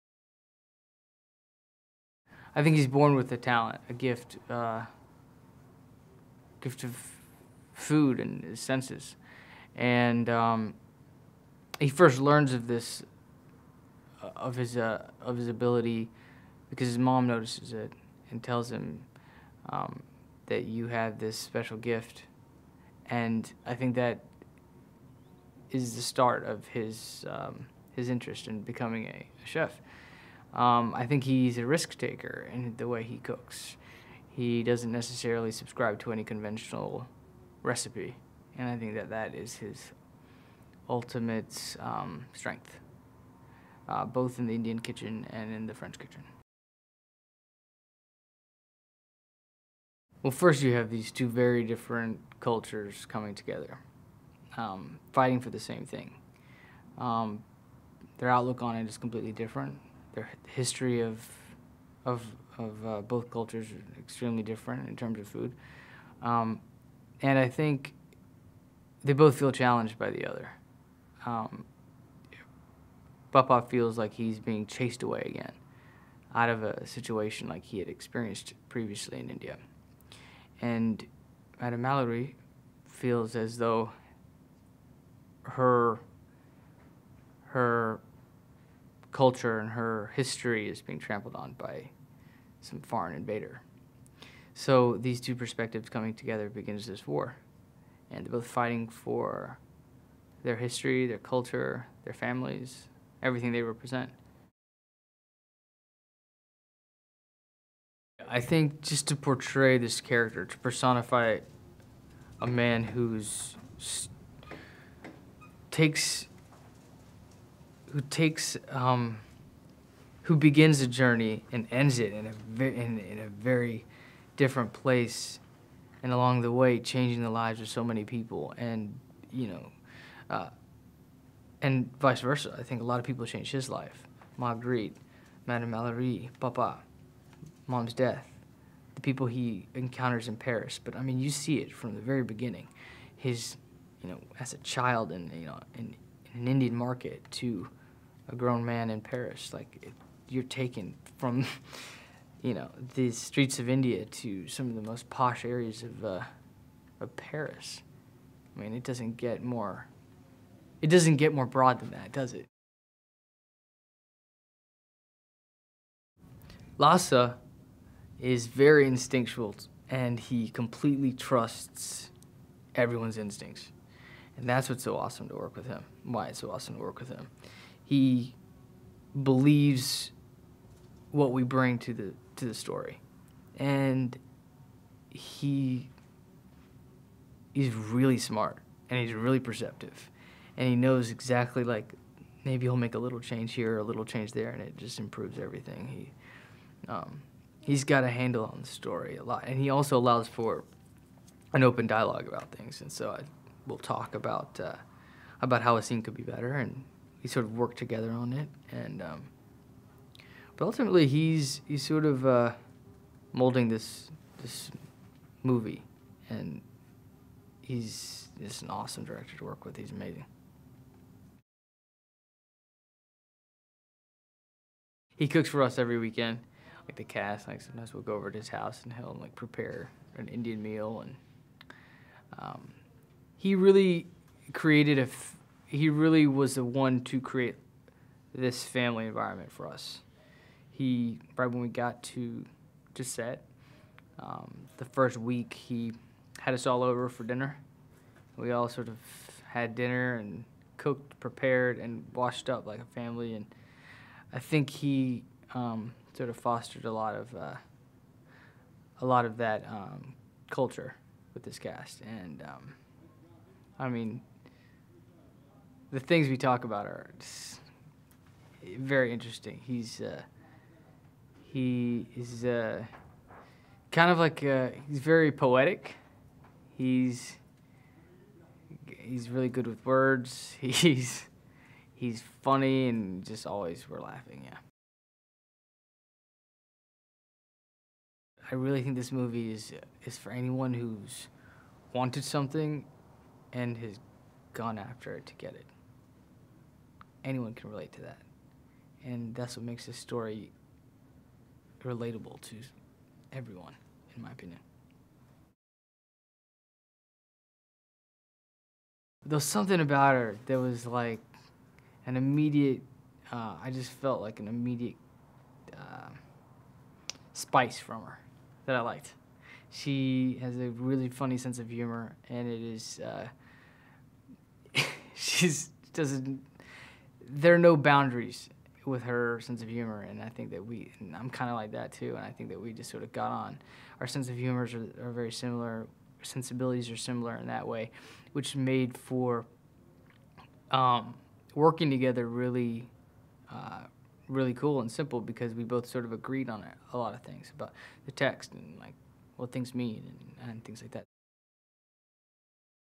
The Hundred-Foot Journey Interview - Manish Dayal (2014) - Movie HD
A story centered on an Indian family who moves to France and opens a restaurant across the street from a Michelin-starred French restaurant.